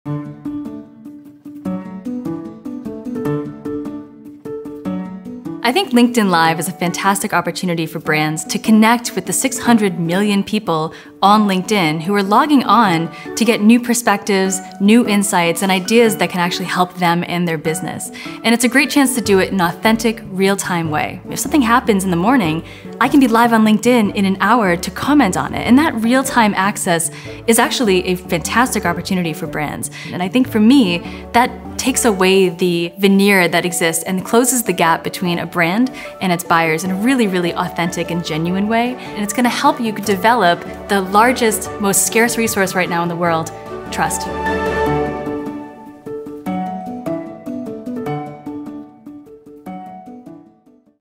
Why Katie Martell Believes LinkedIn Live Helps Brands Develop Trust
Livestreaming on social media can serve a powerful purpose for brands. Katie Martell, our “Unapologetic Marketing Truth-Teller”, explains how creating authentic, real-time content builds trust with business buyers.

Want to test out LinkedIn Live for yourself? Go to lnkd.in/linkedinlive.